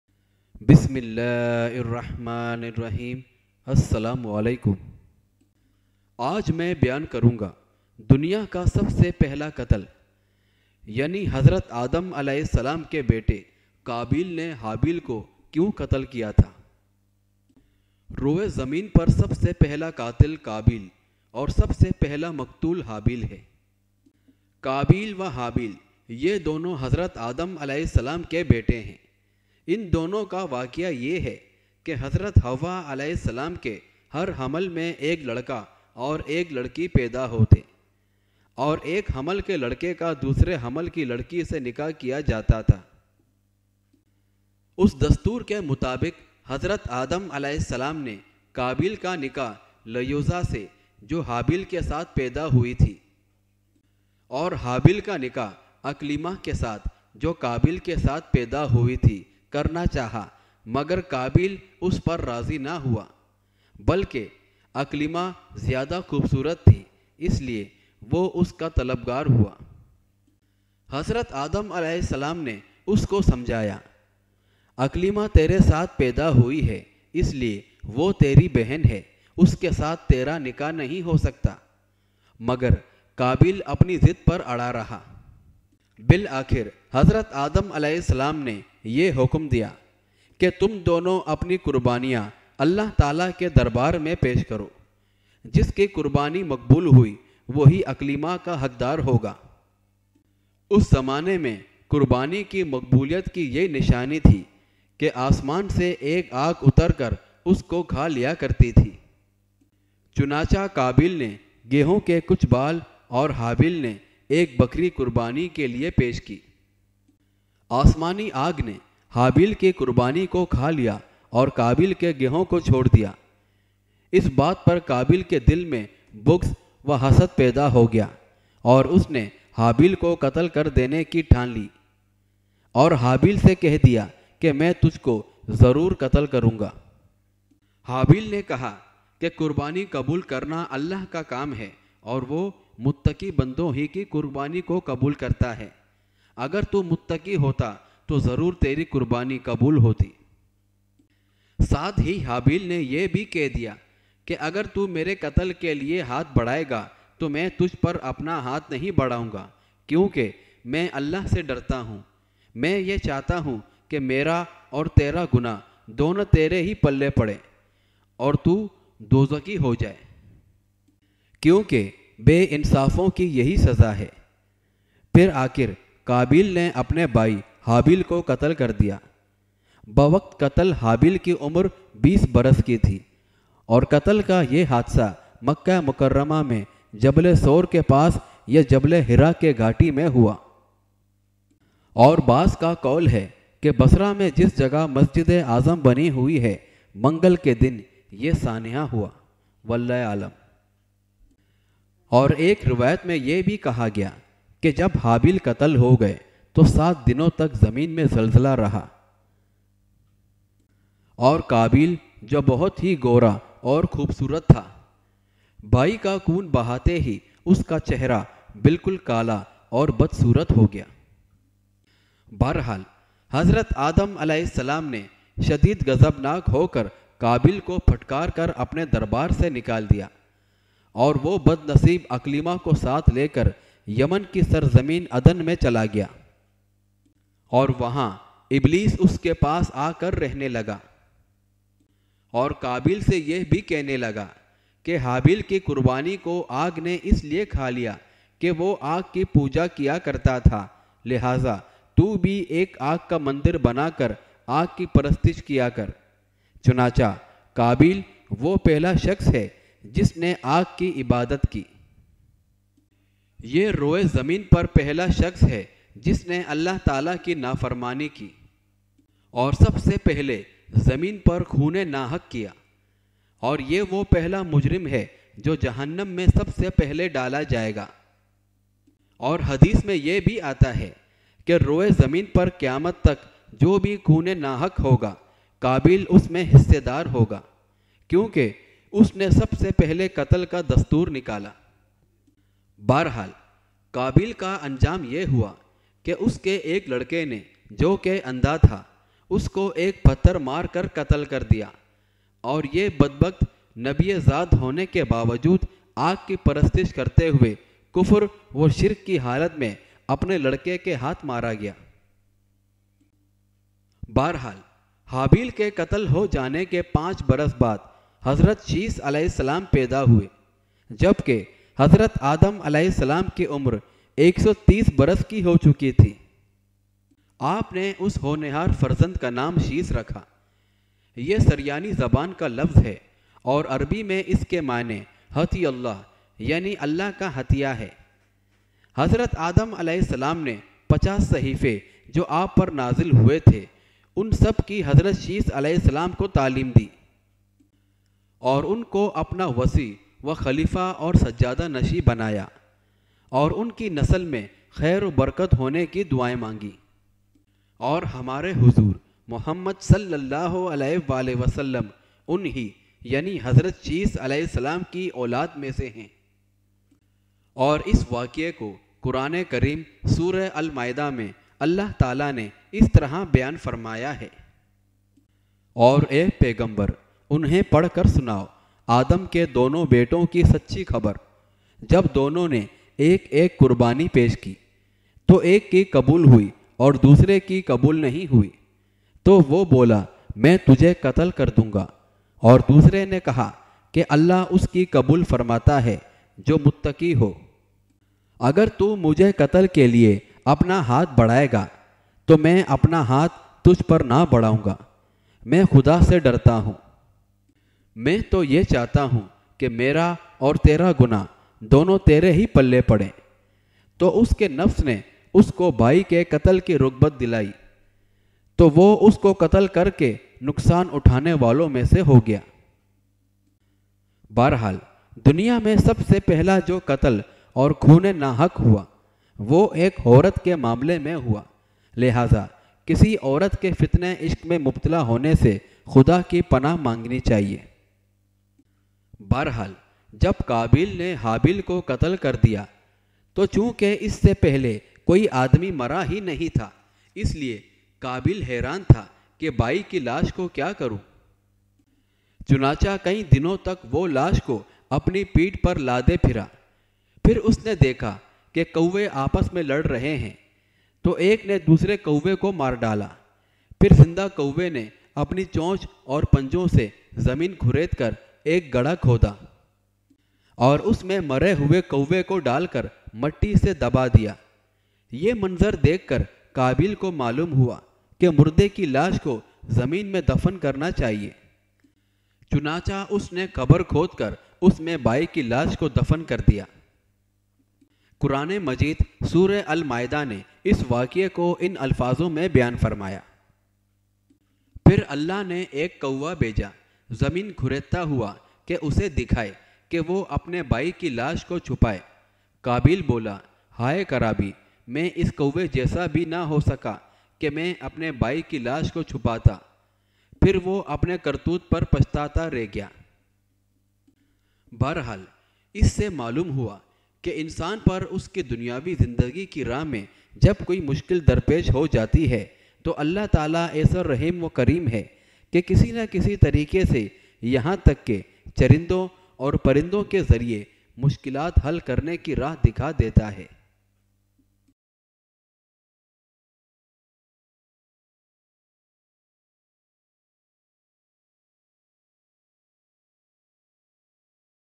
Habeel or Qabeel Ka Qissa | Prophet ADAM (PBUH) | First Murder On The Earth | Akbar Ali Network
Hazrat Adam (AS) Ke Bete Habeel Or Qabeel Ka Waqia | Duniya Main Sab Se Pehla Qatal | دنیا کا پہلا قتل ہے۔
حضرت آدم کے بیٹے ہابیل اور قابیل کا قصہ